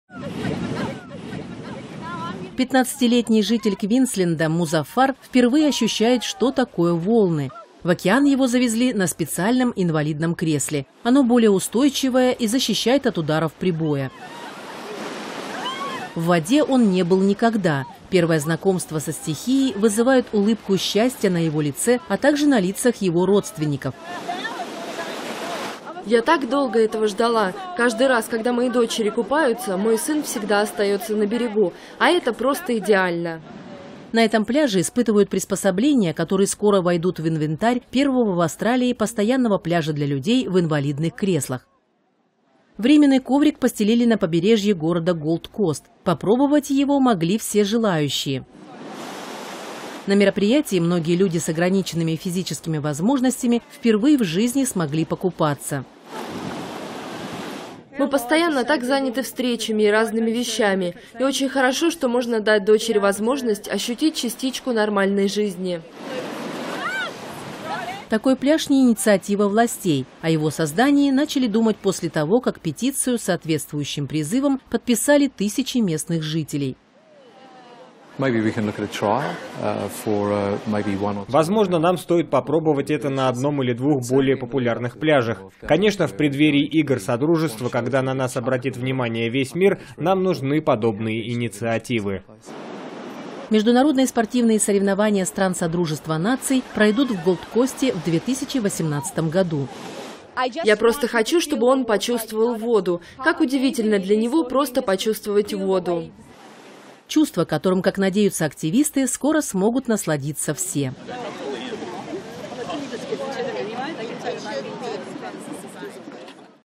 В Австралии создают пляж для людей в инвалидных креслах (новости)
В Австралии создают пляж для людей в инвалидных креслах. 15-летний житель Квинсленда Музаффар впервые ощущает, что такое волны. В океан его завезли на специальном инвалидном кресле. Оно более устойчивое и защищает от ударов прибоя.

В воде он не был никогда. Первое знакомство со стихией вызывает улыбку счастья на его лице, а также на лицах его родственников.

[Ферзина, мать Музаффара]:
«Я так долго этого ждала. Каждый раз, когда мои дочери купаются, мой сын всегда остаётся на берегу. А это просто идеально».

На этом пляже испытывают приспособления, которые скоро войдут в инвентарь первого в Австралии постоянного пляжа для людей в инвалидных креслах.

Временный коврик постелили на побережье города Голд-Кост. Попробовать его могли все желающие.

На мероприятии многие люди с ограниченными физическими возможностями впервые в жизни смогли покупаться.

[Джуди Грин, приехавшая с дочерью-инвалидом]:
«Мы постоянно так заняты встречами и разными вещами. И очень хорошо, что можно дать дочери возможность ощутить частичку нормальной жизни».

Такой пляж не инициатива властей. О его создании начали думать после того, как петицию с соответствующим призывом подписали тысячи местных жителей.

[Боб Ла Кастра, член городского совета Голд-Коста]:
«Возможно, нам стоит попробовать это на одном или двух более популярных пляжах. Конечно, в преддверии Игр Содружества, когда на нас обратит внимание весь мир, нам нужны подобные инициативы».

Международные спортивные соревнования стран Содружества наций пройдут в Голд-Косте в 2018 году.

[Ферзина, мать Музаффара]:
«Я просто хочу, чтобы он почувствовал воду. Как удивительно для него просто почувствовать воду».

Чувство, которым, как надеются активисты, скоро смогут насладиться все.